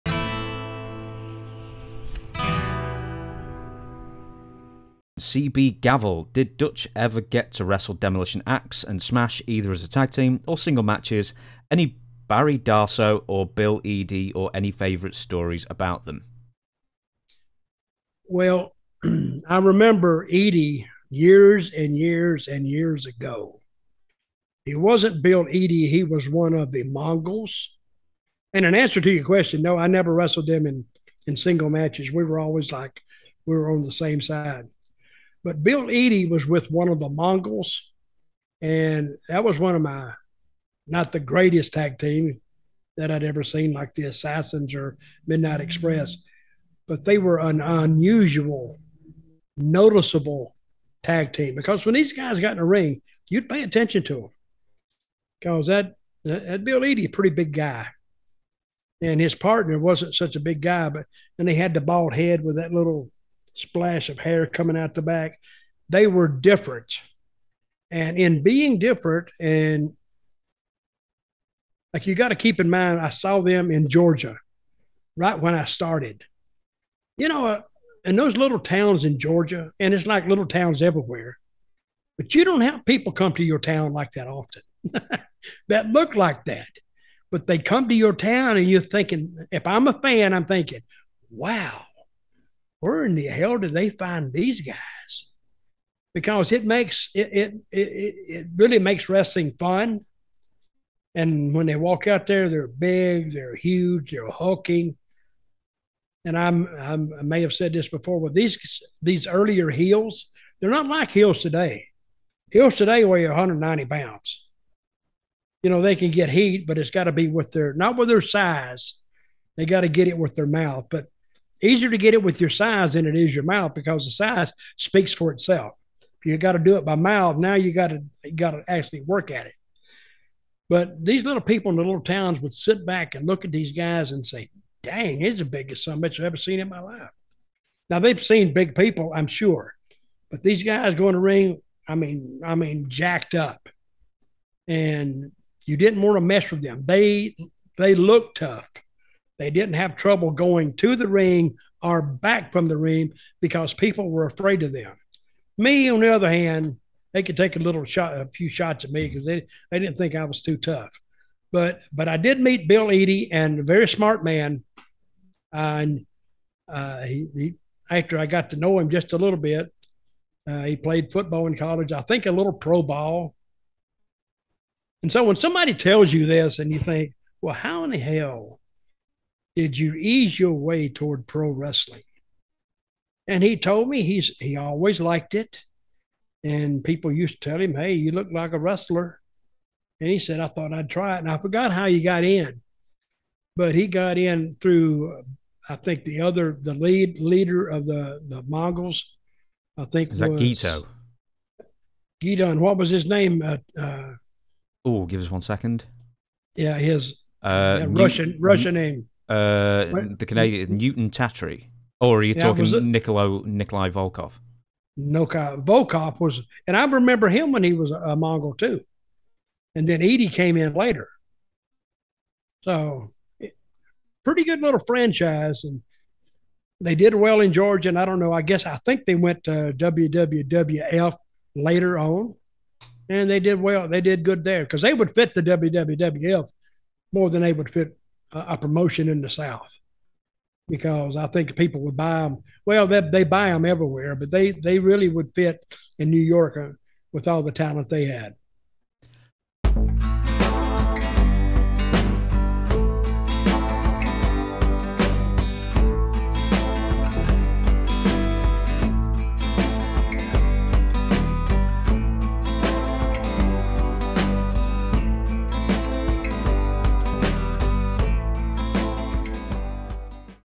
Dutch Mantell on Demolition Ax (Bill Eadie's) First Gimmick - Bolo Mongol
Dutch Mantell on Bill "Demolition Ax" Eadie's First Gimmick - Bolo Mongol